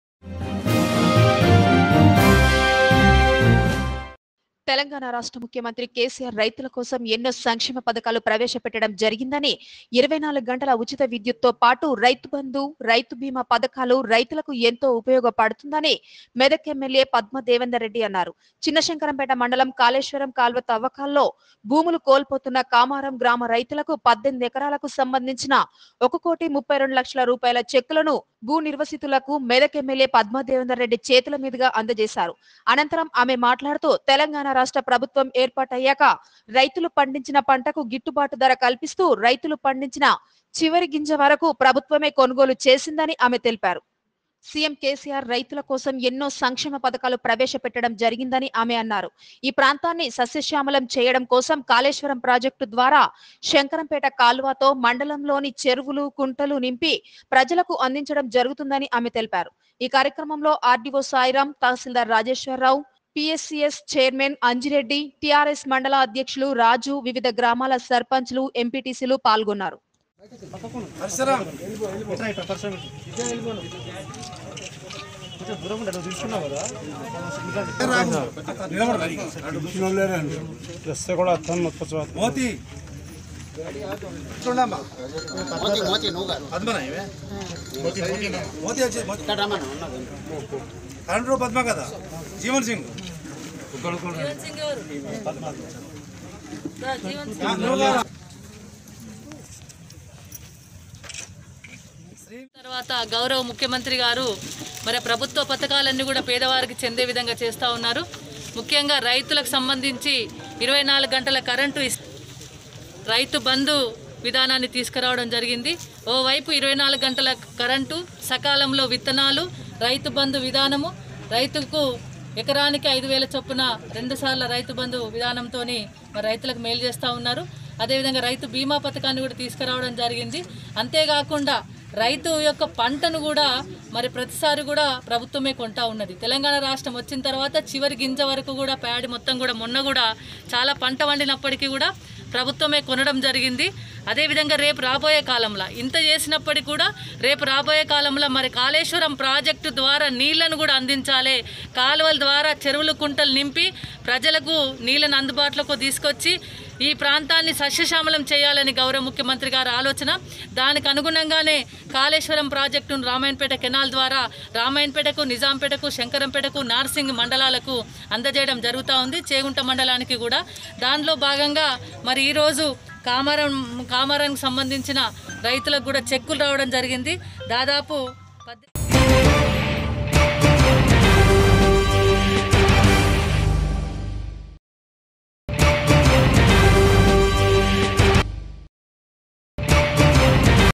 భూములు కోల్పోతున్న కామారం గ్రామ రైతులకు కోటి 32 లక్షల రూపాయల చెక్కుల అందజేత || Nethra News
|| Nethra News || చిన్న శంకరం పేట మండలం ||